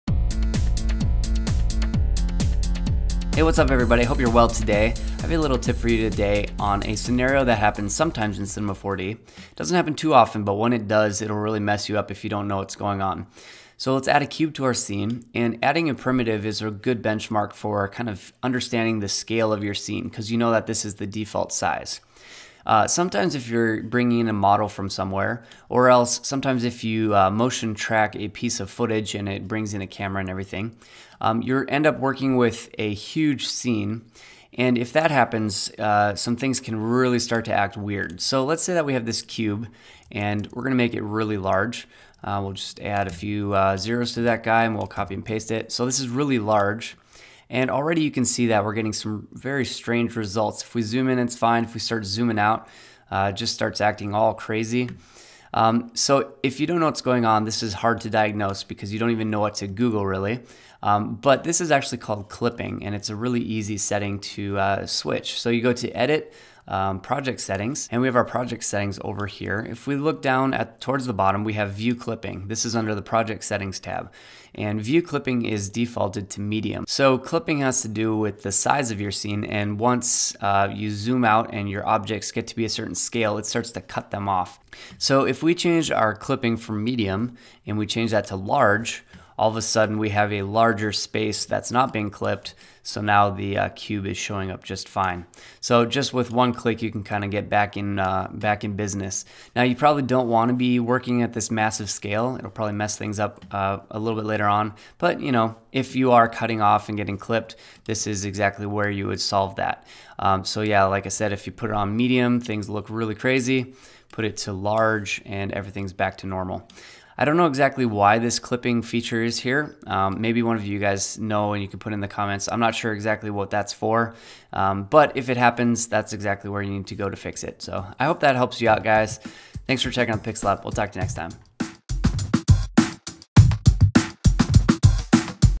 C4D Quick Tip: Objects in Cinema4D Getting Cut Off or Clipped
If you have ever had weird results with a model or tracked scene getting cut off or acting strangely it might be your clipping settings. In this quick tip I'll show you how to solve this issue. It shows up every once in awhile, so it's good to know about for future reference!
For more free training and 100+ free models go to thepixellab.net